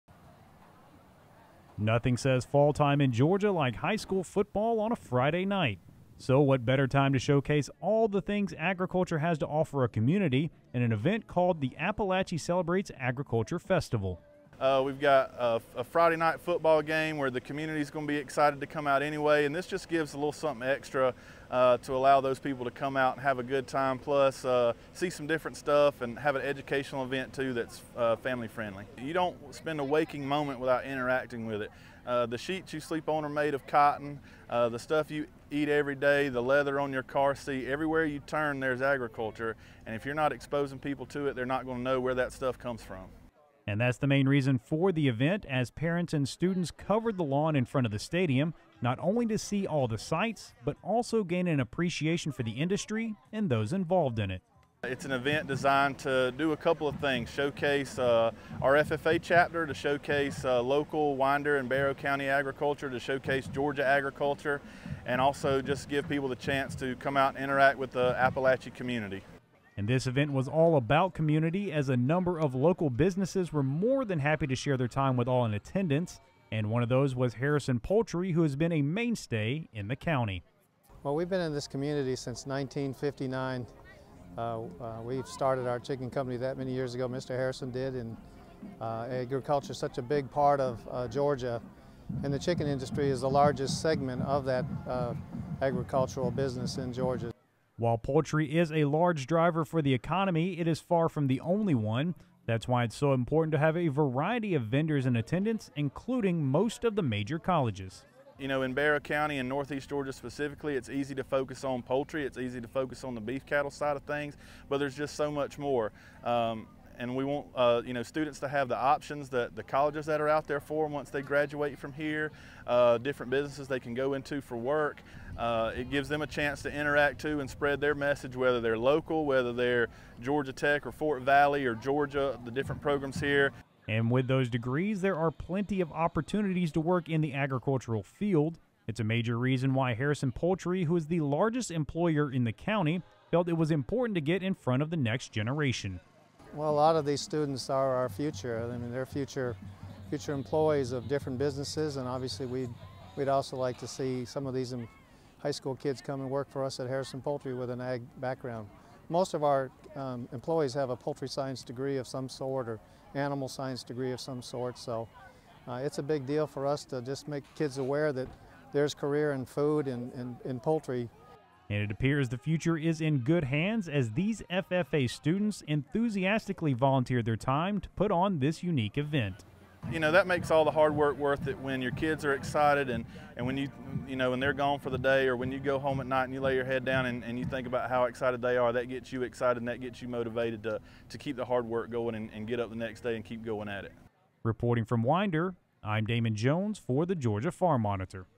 Agriculture and Football Celebrated in Barrow County, Georgia
Football and agriculture are two things the state of Georgia takes very seriously. recently, the Apalachee High School FFA celebrated both by holding the inaugural Celebrate Agriculture Festival… just before the Wildcats hit the field. Damon Jones has the story from Barrow County, Georgia.